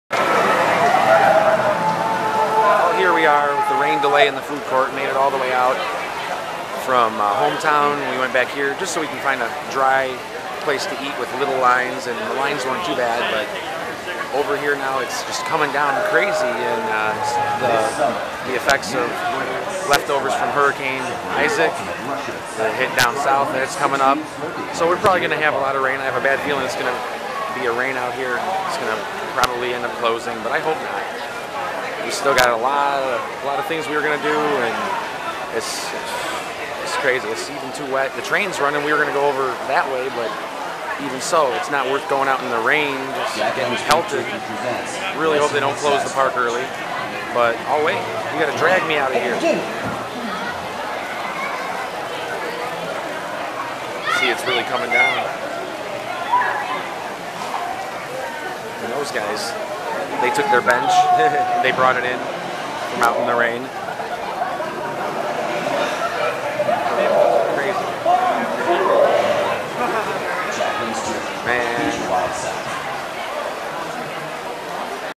Food Court Rain Delay Six Flags Great America 9-1-12
After leaving Southwest Territory, aerohead4 and I made it to Hometown Square so I could get my dining passes. If there's a rain delay, I might as well get lunch out of the way and see what happens. Any dry areas to eat were crowded, so we ended up at the food court in County Fair. I ended up standing eating my chili cheese dog on a counter of a closed cash register, while others who weren't eating were occupying the covered tables. Selfish people, I tell you!